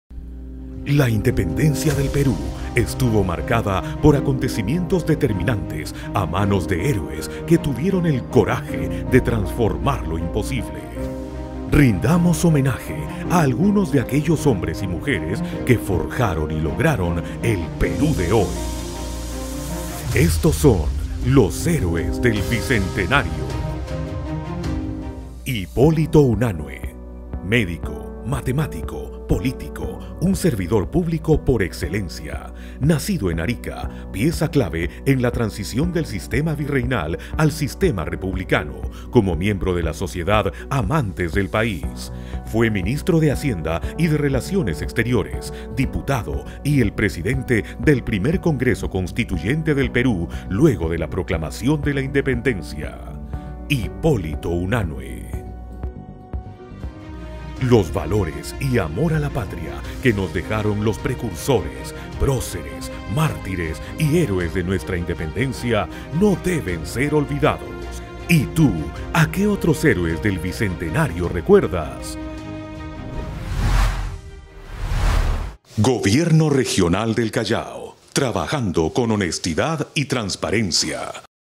Callao es Bicentenario | Homenaje a Hipólito Unanue
Hoy rendimos homenaje a Hipólito Unanue, un servidor público por excelencia, pieza clave en la transición del sistema virreinal al sistema republicano. Conoce más de este Héroe del Bicentenario en este video. 👇🏽